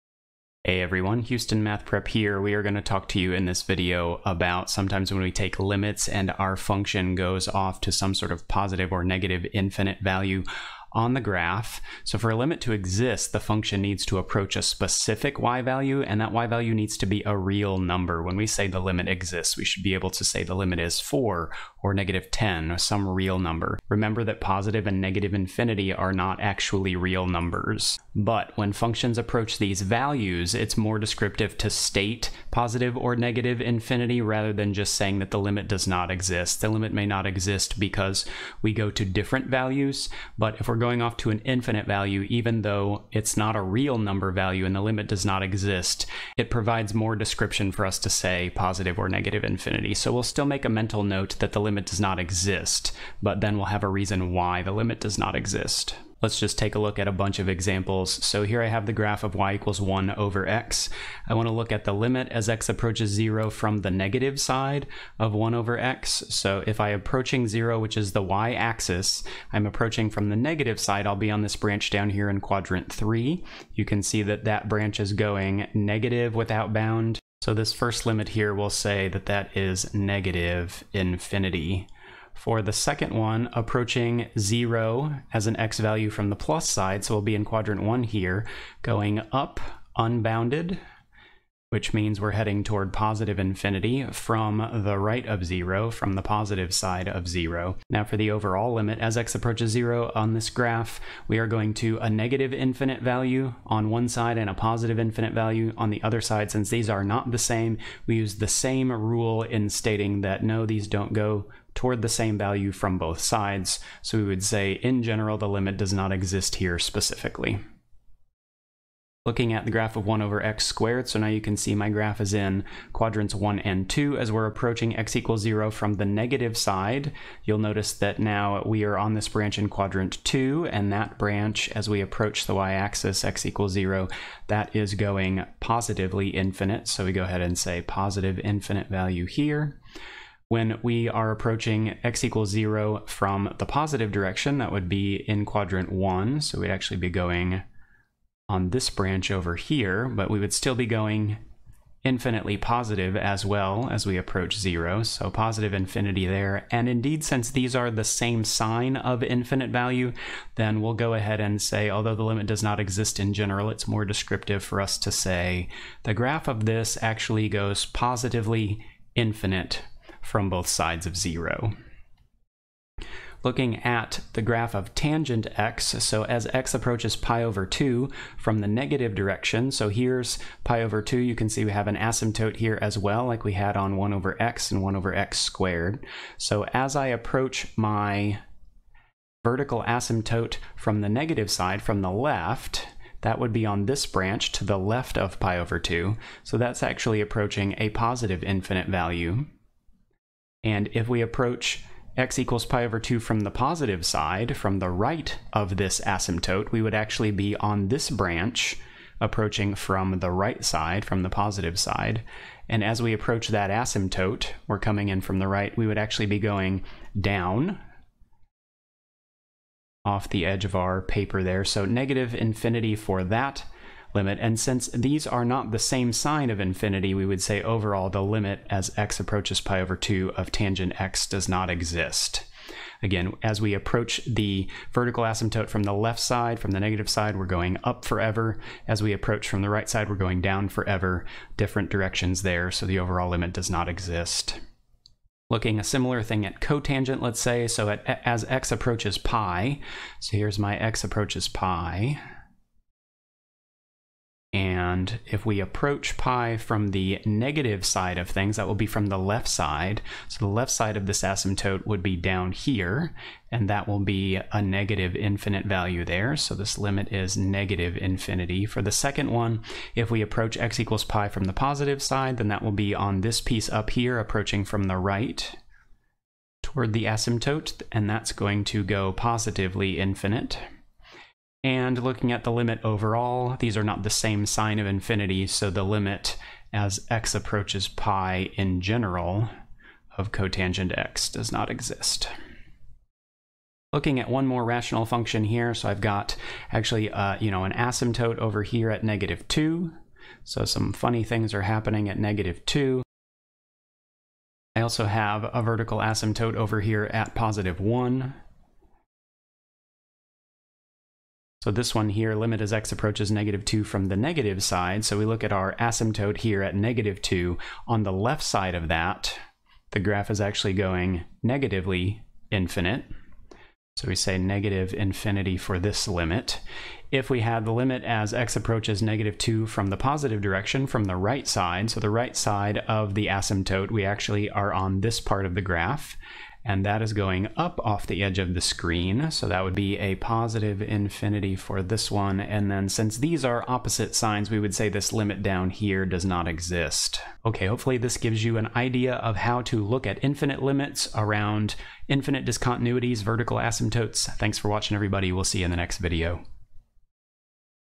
Infinite Limits (Calculus 1)
This Calculus 1 video on infinite limits explains how it is better to describe a limit as infinite, if we approach a vertical asymptote (infinite discontinuity), rather than to just say that the limit does not exist. We work several examples from a graph that include rational functions and trigonometric functions (tangent and cotangent). Our examples include one-sided limits and overall limits.
0:00  Introduction to infinite limits
0:56  Example 1 (Rational function)
1:59  Example 2 (Rational function)
2:53  Example 3 (Trig function - Tangent)
4:05  Example 4 (Trig function - Cotangent)
4:53  Example 5 (Rational function)